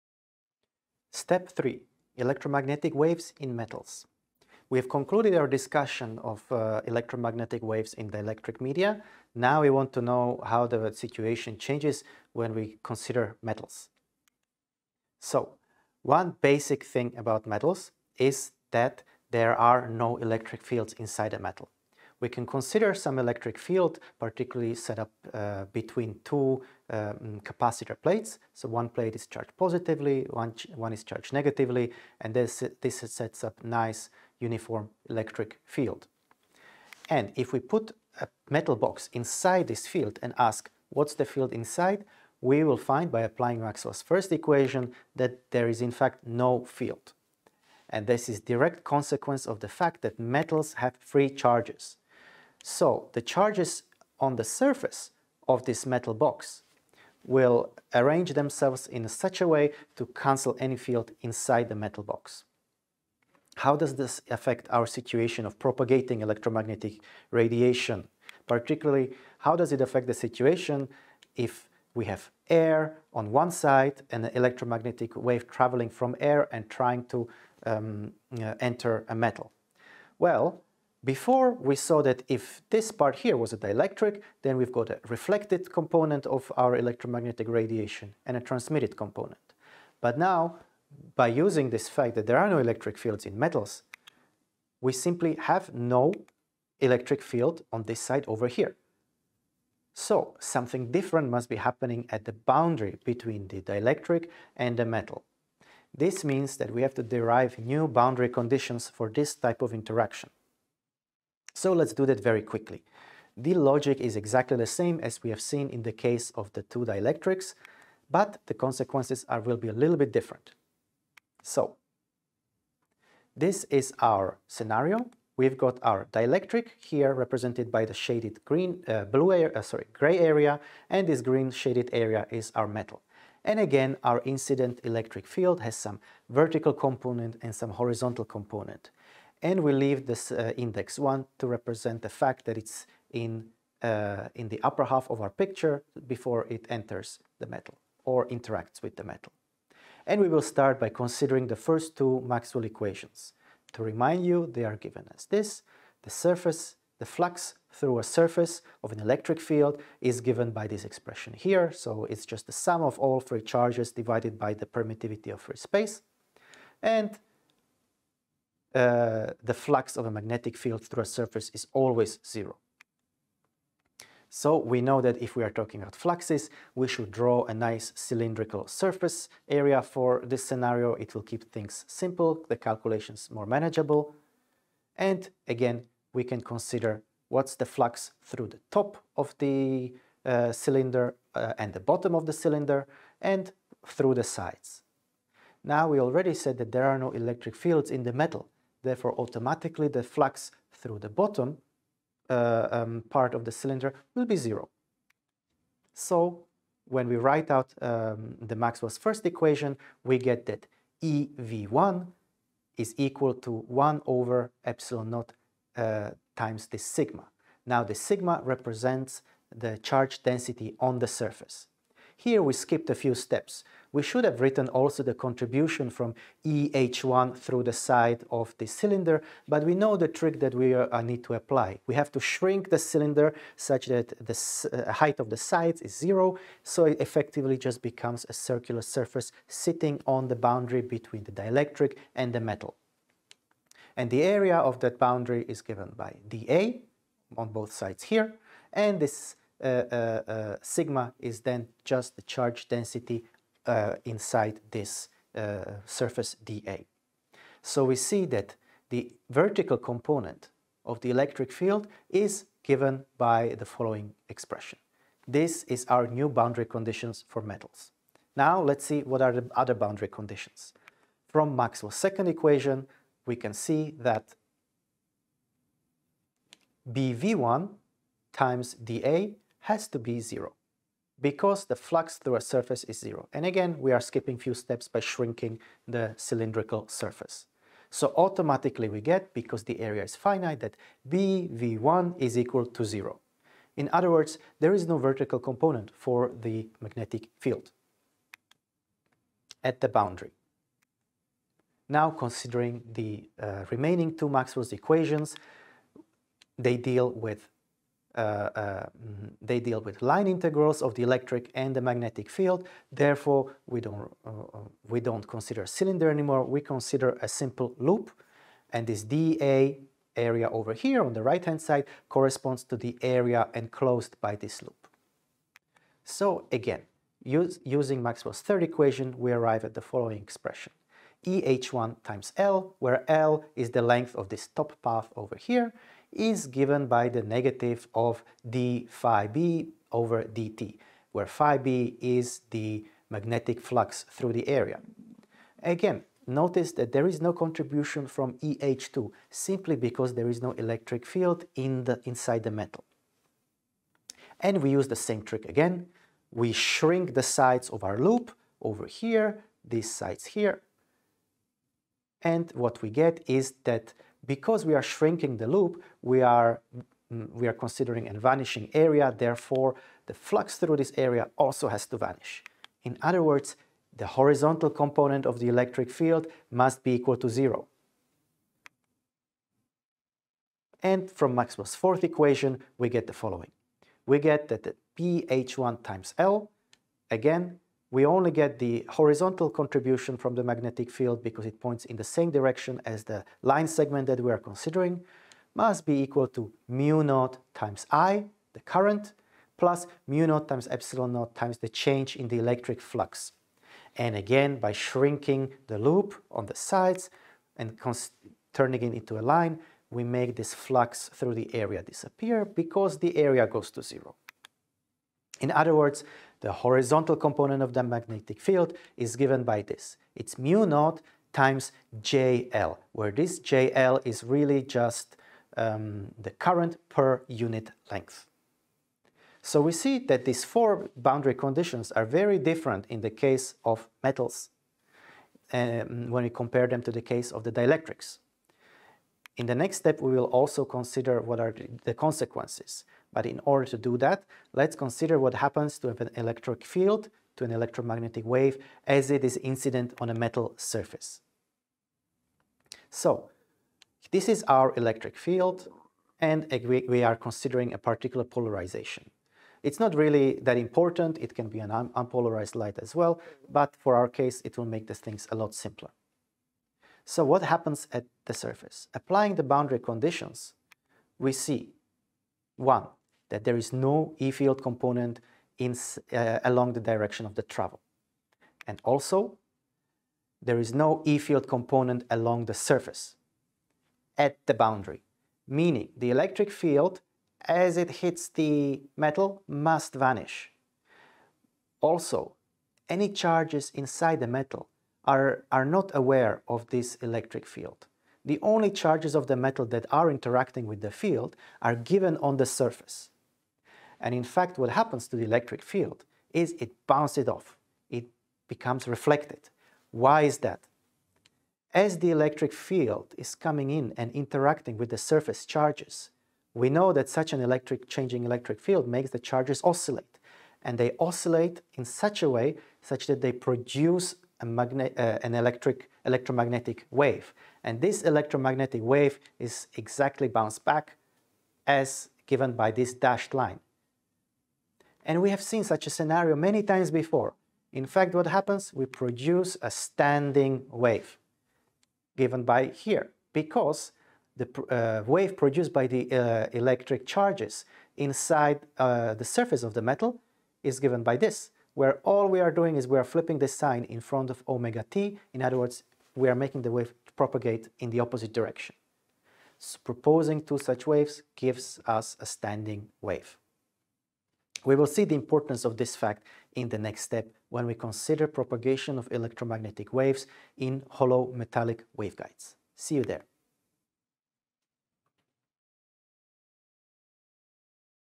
9-3 EM waves and metals
Lesson 9 EM Waves in Optical Media II
Step 3: EM waves and metals
In this step, we learn about the differences in how light behaves when incident on a metal surface rather than a dielectric material.

This is part of our module: "From Classical to Quantum Light".